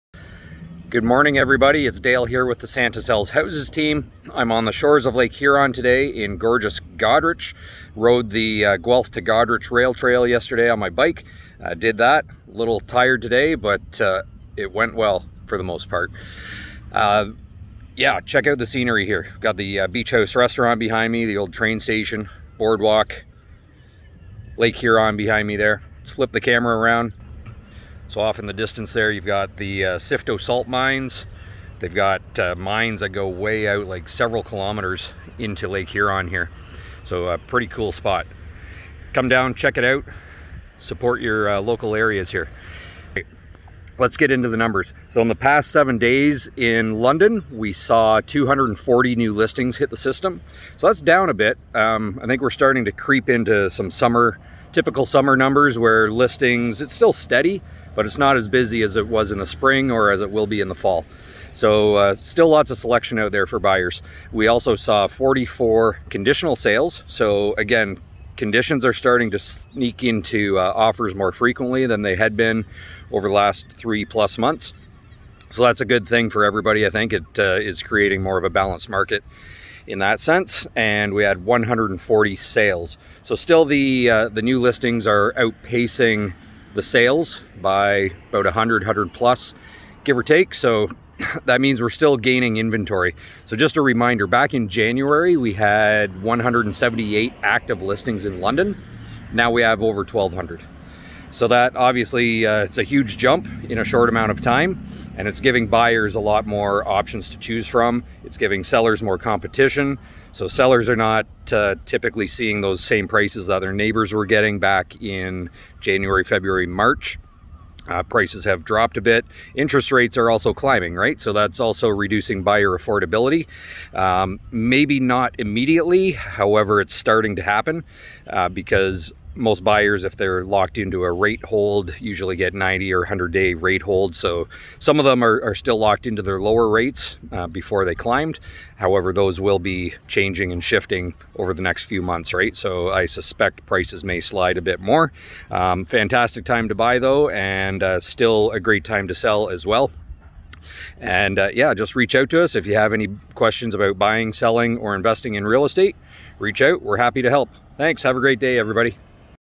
Monday Market Update (Episode 279)
Dale is joining us from the shores of Lake Huron in Goderich for this weeks Monday Market Update 🌊 . He’s here to let us know that we’re finally seeing some Summer market conditions. With new listings high, and inventory even higher, we can expect that the market is creeping closer to being balanced. What does this mean for you? Tune in & find out more.